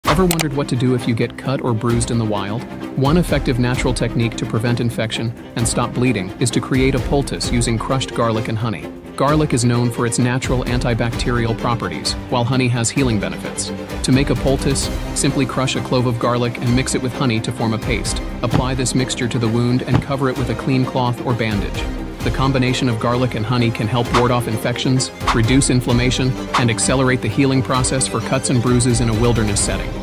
Natural First Aid in the Wild: Garlic and Honey Poultice for Cuts and Bruises
Learn how to use a simple yet effective natural remedy in the wilderness to prevent infections and stop bleeding. Crushed garlic and honey combine to create a healing poultice that can accelerate the healing process for cuts and bruises. Follow easy steps to make and apply this poultice for quick relief and protection in outdoor emergencies.